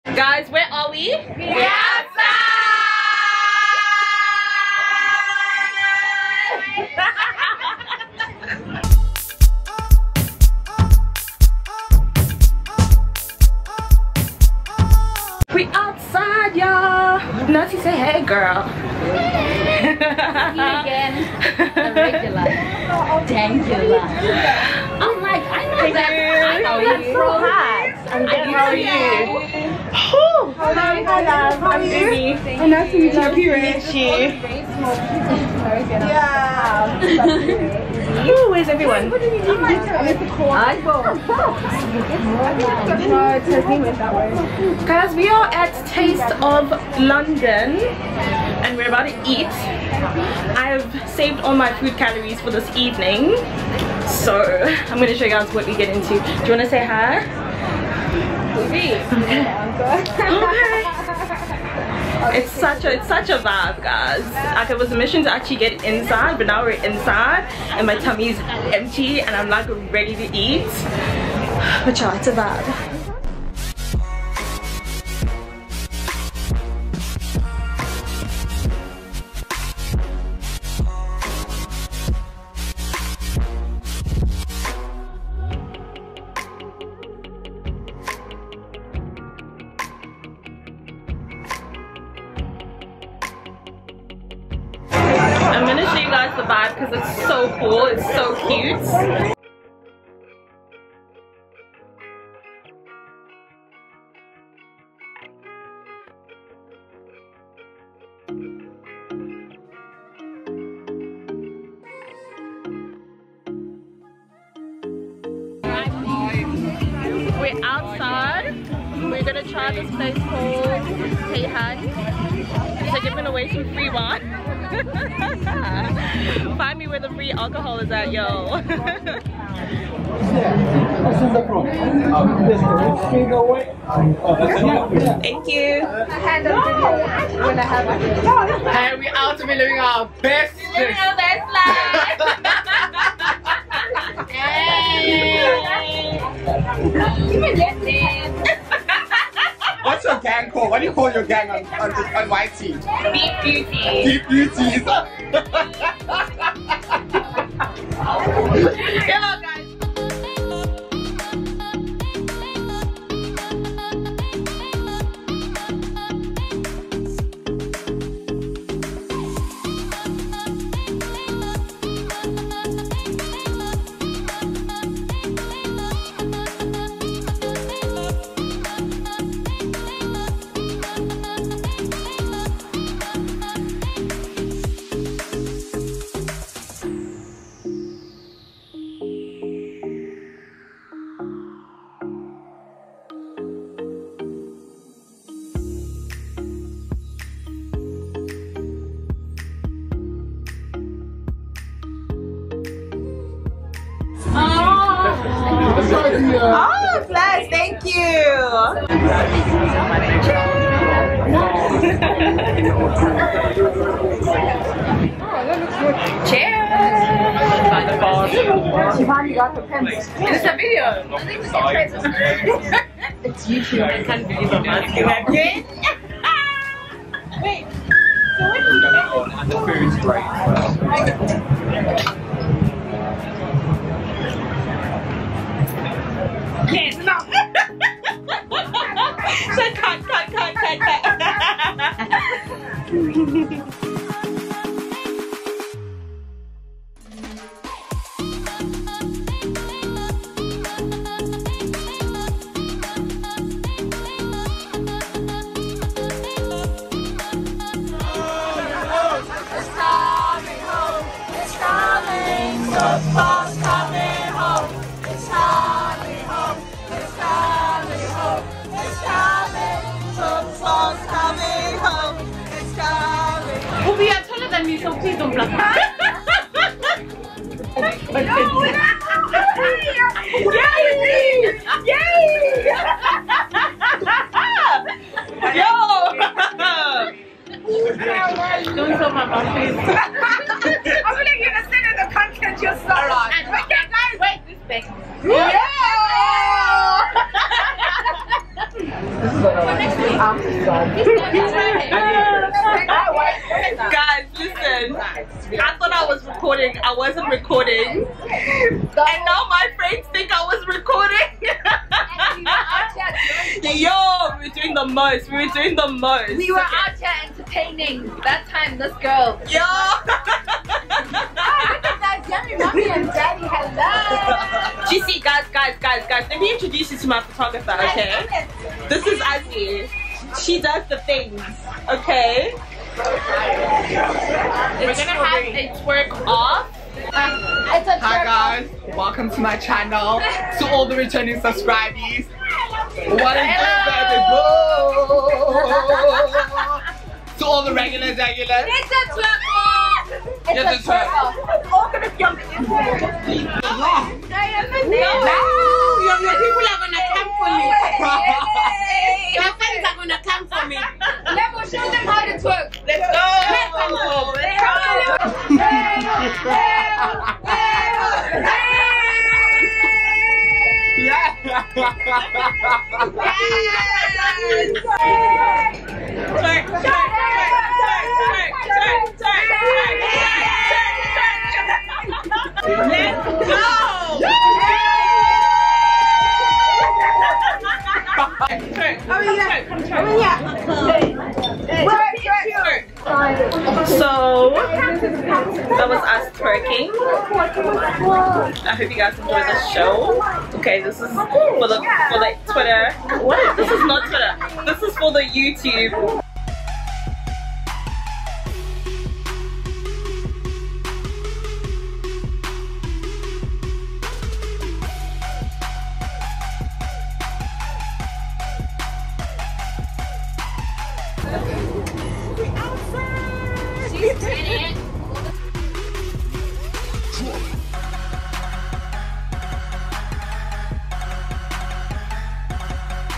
LONDON VLOG TASTE OF LONDON FOOD FESTIVAL | CA SA IN LONDON
Watch in 1080HD - please don't skip ads 🥺

Hey lovelies!! I cannot express how much fun we had at the Taset of London Food Festival. I didn't even get as much footage as I'd normally get because we were so busy with the food and just enjoying the opportunity to be outside. We are a bunch of Chartered Accountant (South Africa) who have relocated to Europe (London and Ireland) and we got to enjoy the fruits of our labour a little bit. I hope you enjoy it the vlog as much as we enjoyed the festival ♥

EQUIPMENT
Canon 70D
Canon M10 with Canon 15-45mm lens

May the Lord cause you and yours to flourish - Psalm 115:14
ox

(Music used does not belong to me)